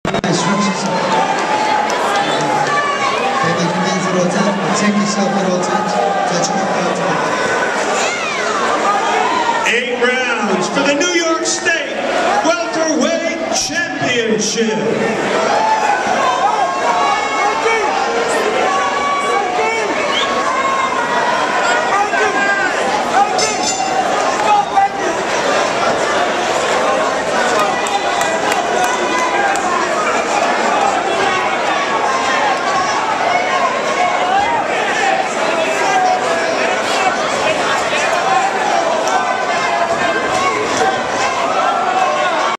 BOX NYC: Ruiz/Wright Opening Bell
BOX NYC main event opening bell:  Mike Ruiz and Martin Wright.